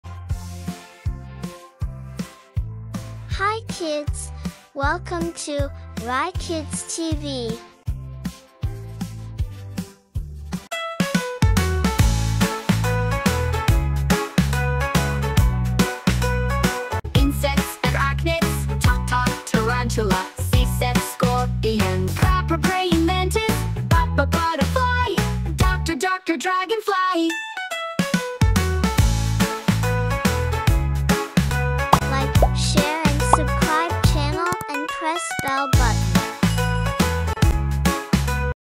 Incredible Insects for Kids | Learn Bug Names and Fun Facts!
Dive into the amazing world of insects! In this video, kids will learn the names of different bugs, including beetles, butterflies, ants, and more. Perfect for young explorers who love creepy crawlies!

insects, insect names, kids learning, fun facts, bugs, beetles, butterflies, ants, learning for kids, educational video